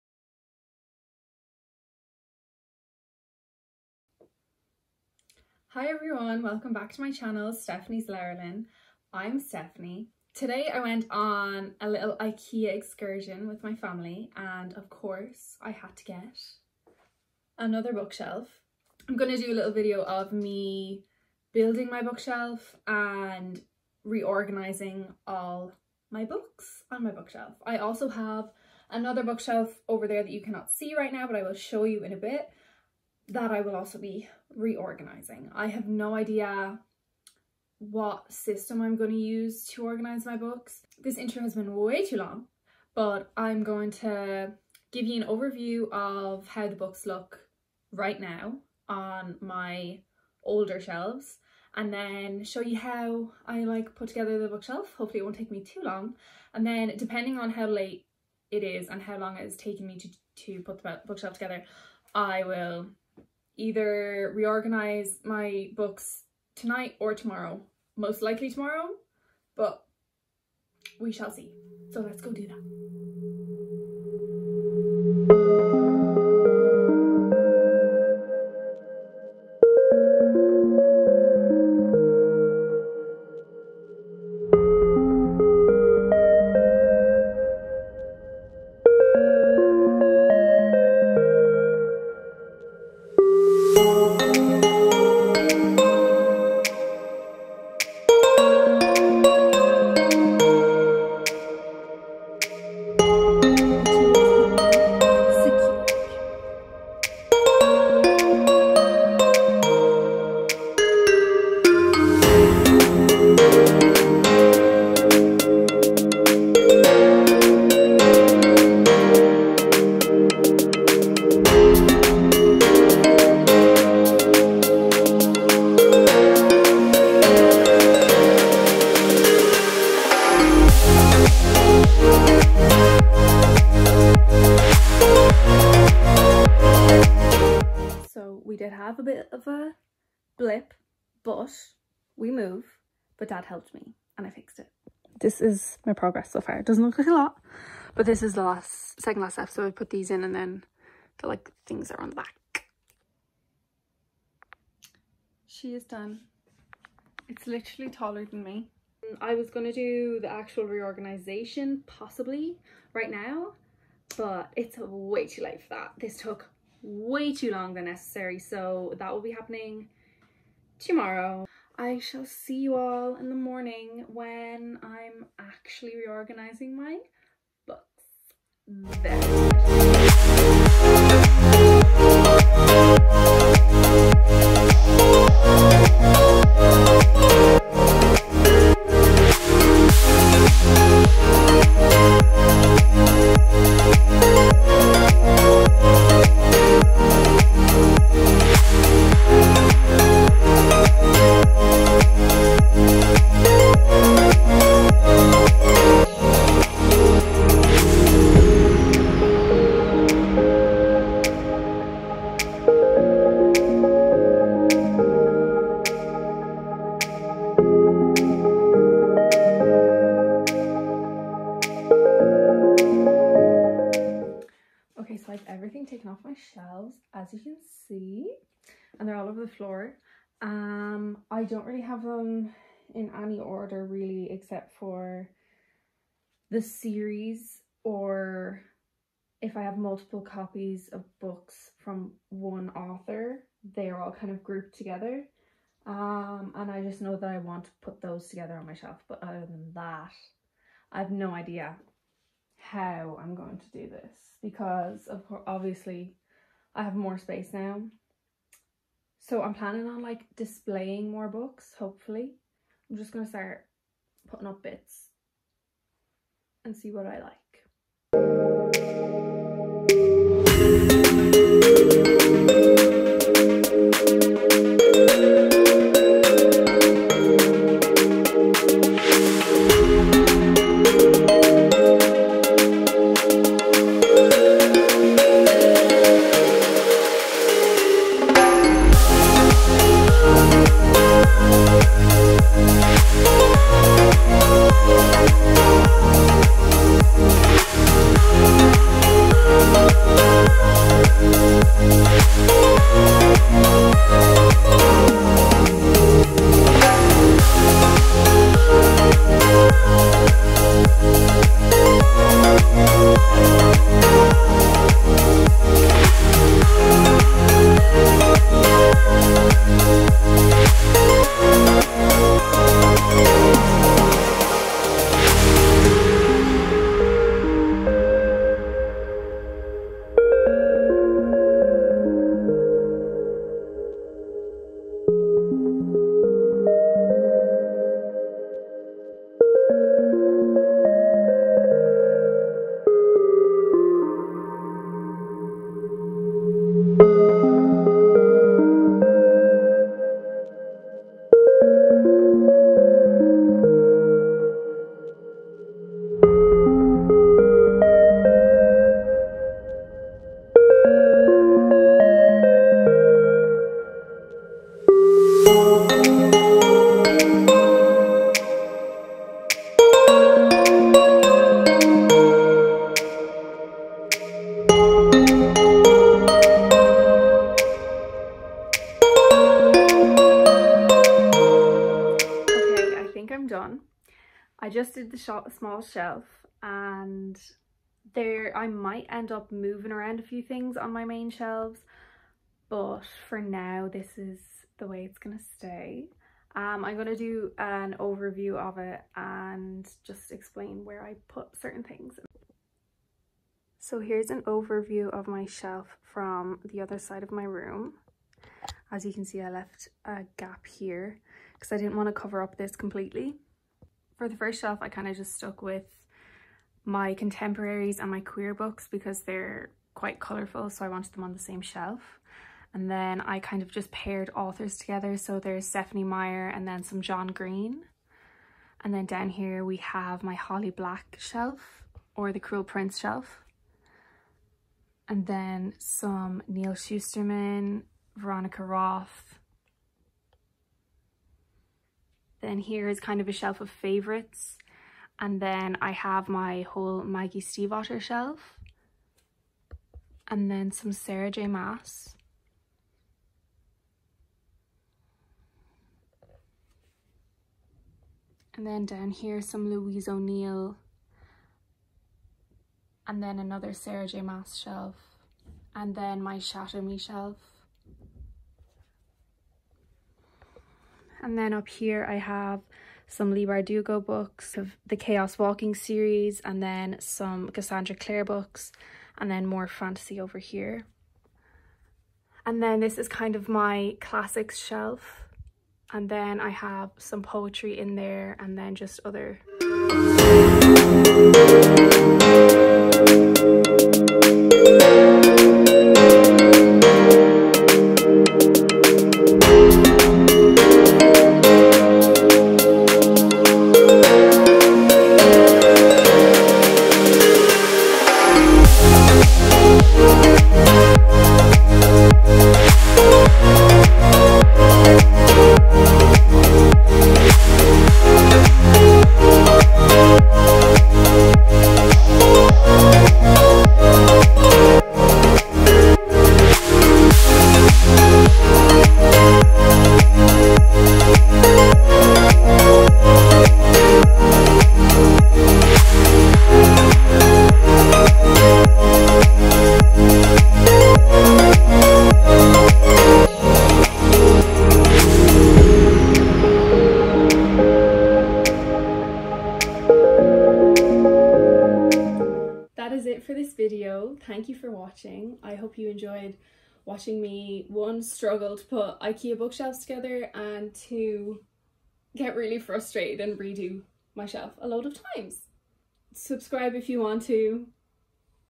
building a new bookshelf & bookshelf reorganization!
welcome back to my channel! this video is a short one where i build a new bookshelf and reorganize all my other shelves too. i hope you enjoy and thank you so much for watching!!